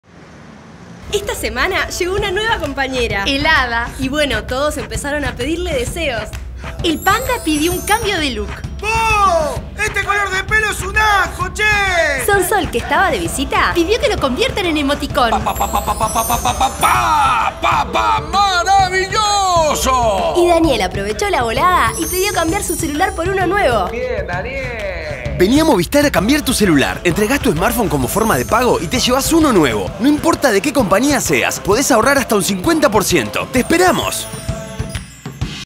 Traé tu Smartphone - Movistar
¡Ahora podés traer tu celular usado de cualquier compañía y cambiarlo por un nuevo 4G!